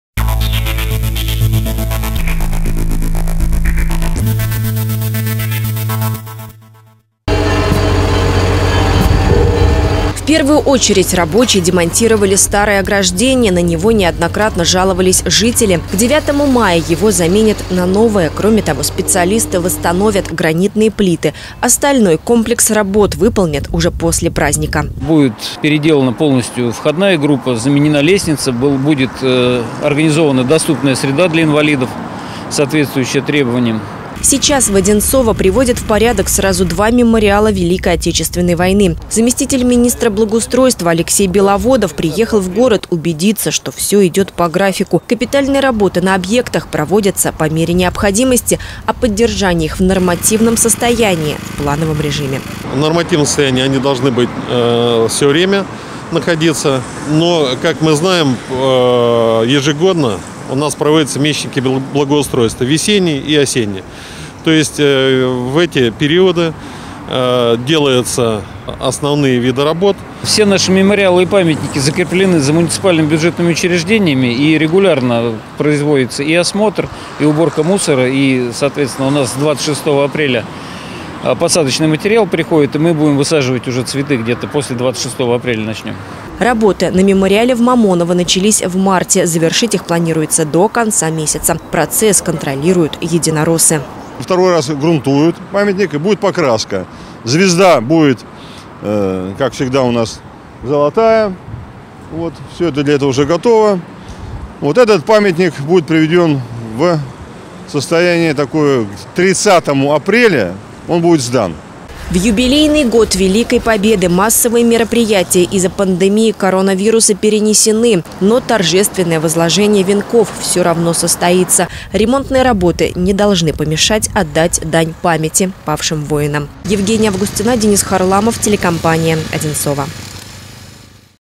Историческая память
Мемориалы Великой Отечественной войны закреплены за эксплуатирующими организациями, которые в плановом порядке следят за их состоянием. Ремонт проводится по мере необходимости. Сейчас работы идут на двух памятниках в Одинцово. Когда они будут завершены, узнала Евгения Августина.